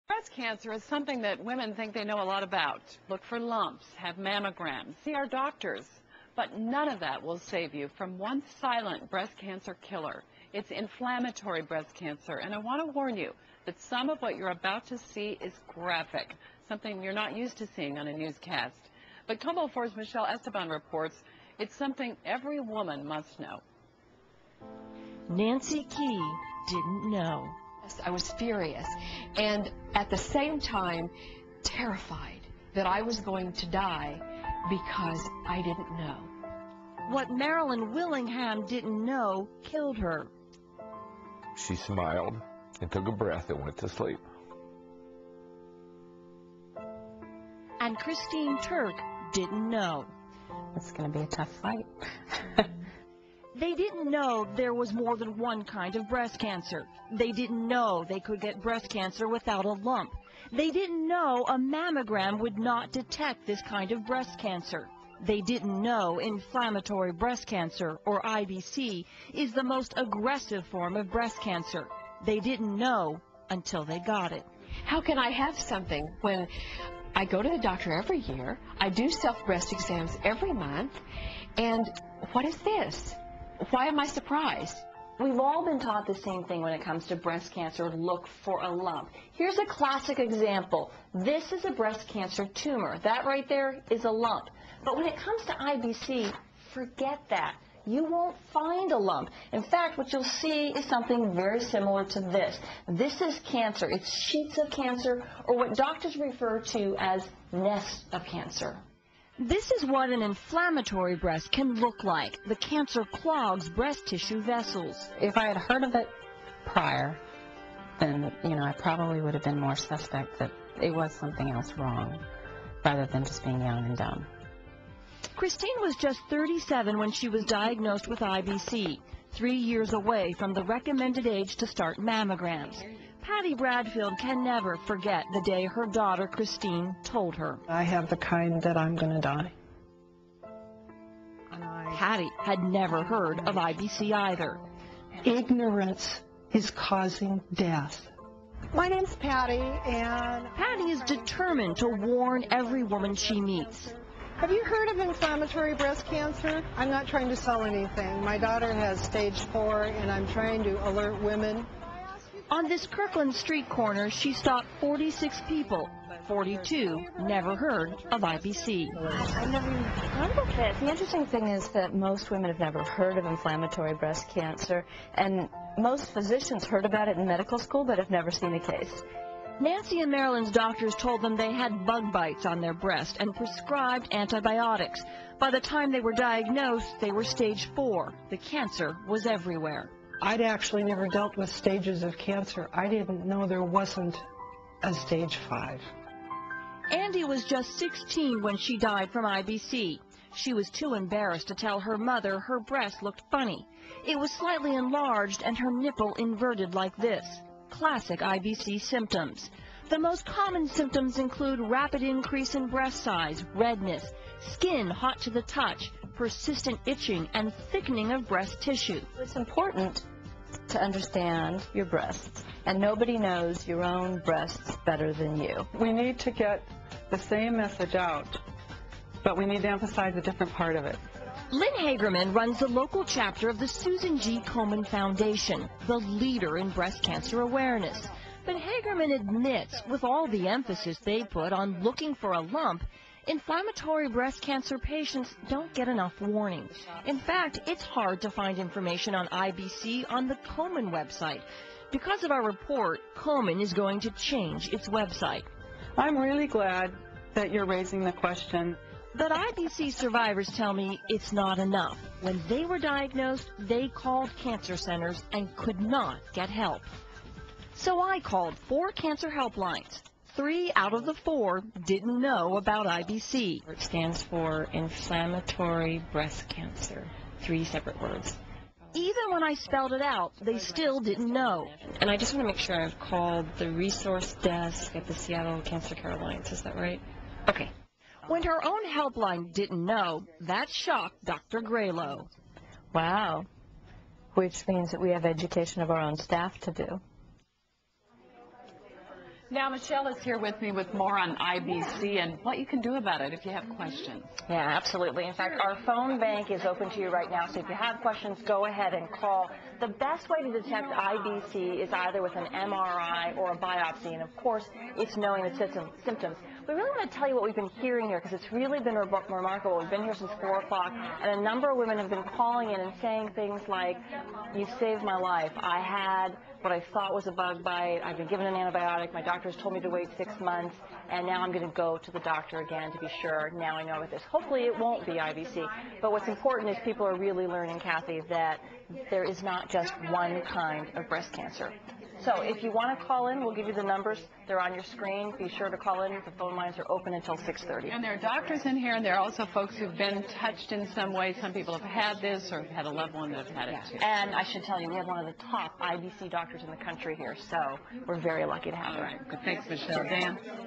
Inflamatory Breast Cancer
This vid contains vital information about a kind of breast cancer that is not easy to spot. Please watch and pass the word it could save a life.